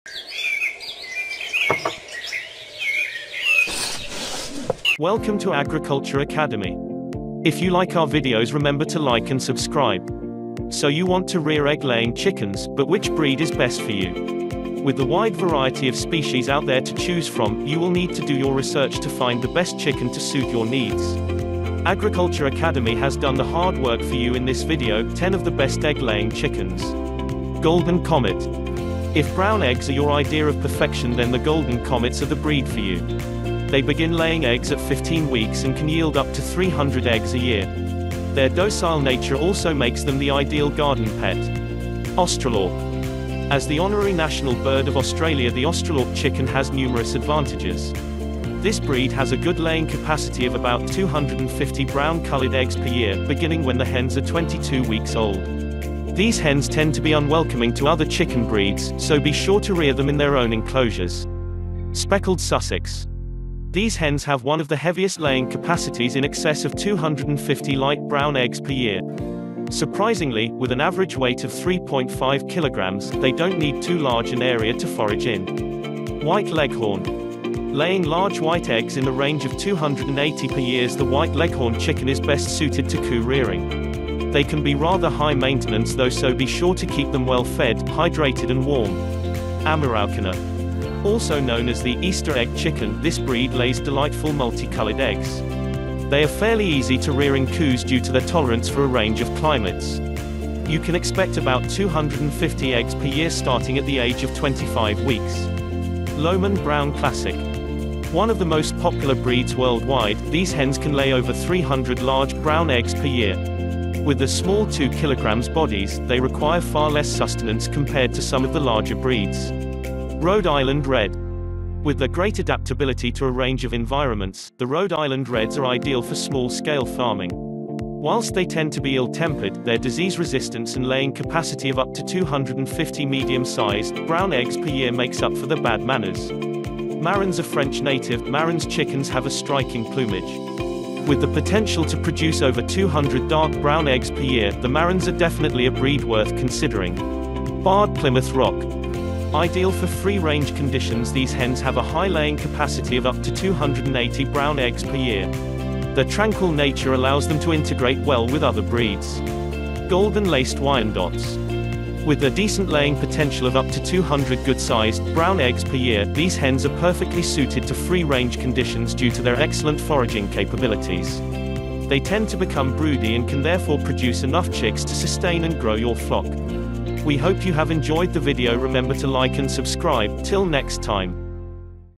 BEST CHICKENS FOR LAYING EGGS.
So you want to rear egg laying chickens? But which breed is best for you? With the wide variety of species out there to choose from, you will need to do your research to find the best chicken to suit your needs. Agriculture academy has done the hard work for you in this video: The Top 10 best egg laying chickens.

Our list includes:
10. Golden Comet
9. Australorp
8. Speckled Sussex
7. White Leghorn
6. Ameraucana
5. Lohmann Brown Classic
4. Rhode Island Red
3. ?
2. ?
1. ?

🐔 Movable chicken coop
🐔 Nesting pads
🐔 Pet bedding
🐔 Hatchery incubator

This includes, but is not limited to, Camtasia, StoryBlocks, and Artlist.